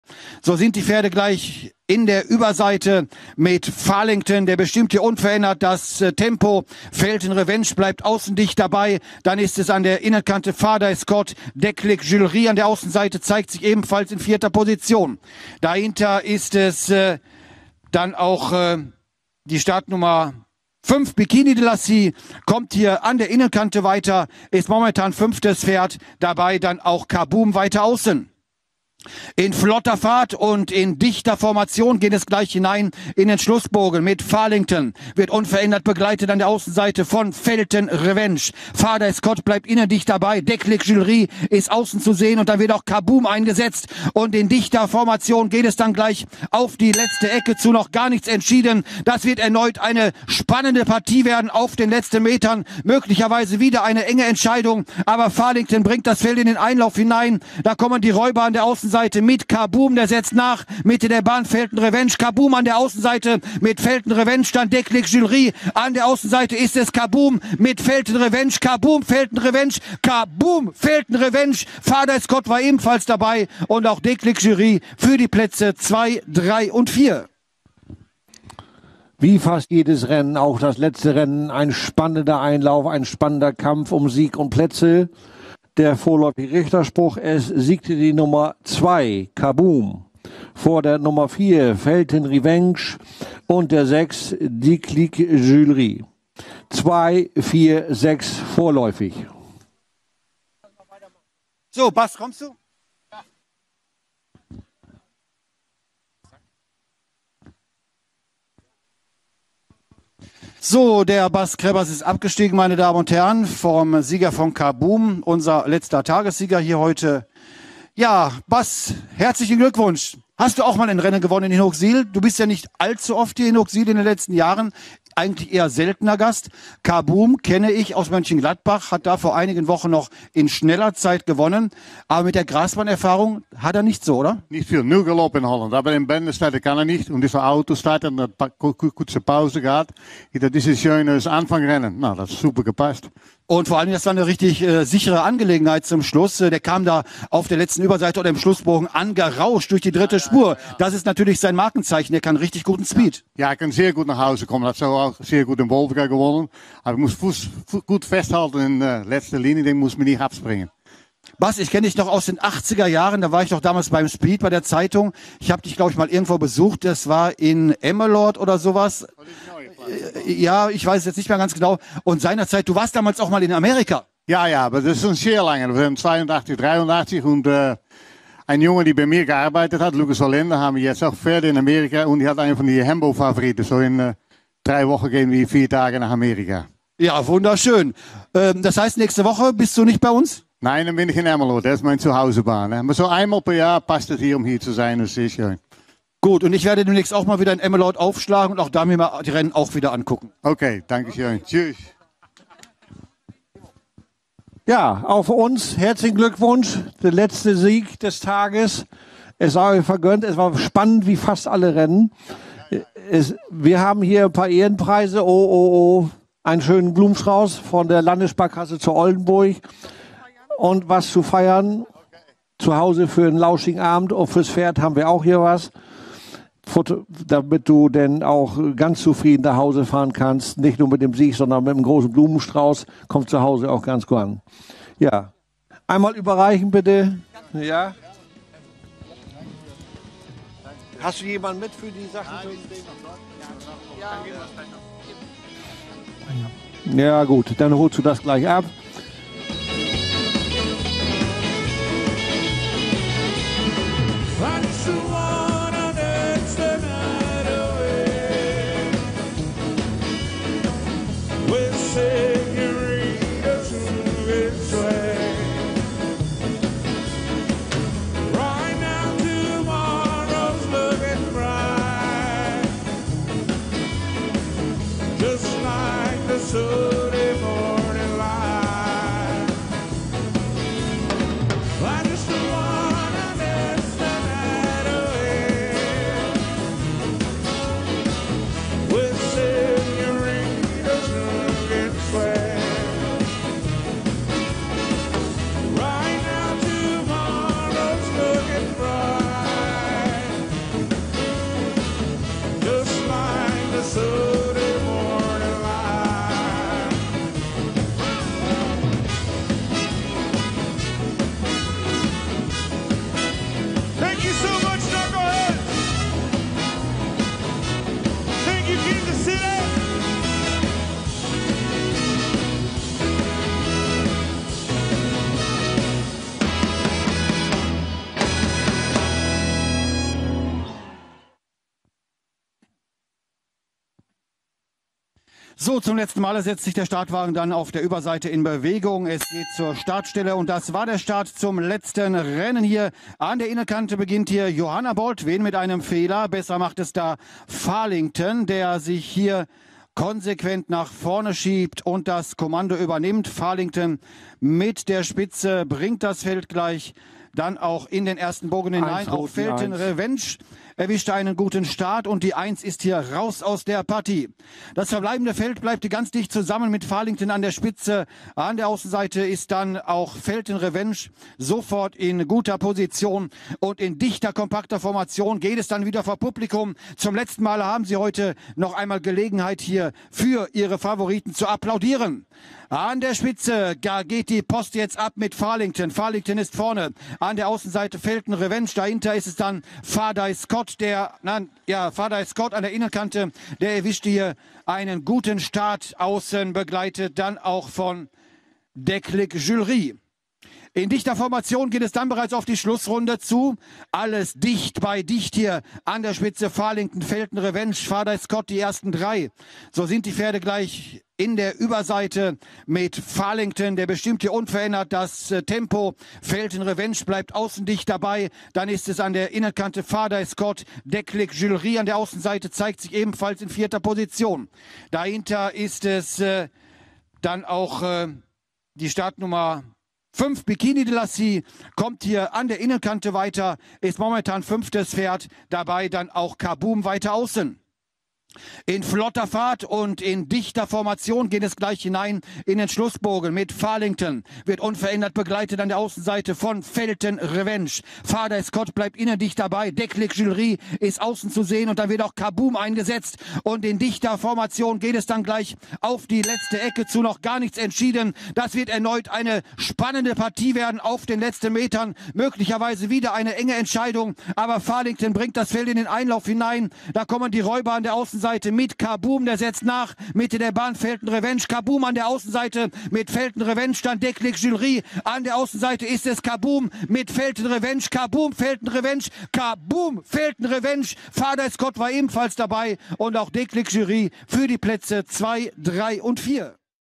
Kaboom & Bas Crebas winnen "Preis der Landessparkasse zu Oldenburg" Hooksiel 13 juli 2022
Kaboom & Bas Crebas winnen "Preis der Landessparkasse zu Oldenburg" voor Payday Racing en tr. Bas Crebas Hooksiel 13 juli 2022
Eig. combinatie: Jim Walraven en Stal Payday - Peter Delis
F.: P. Davidson - H. Zonderman - J. Engwerda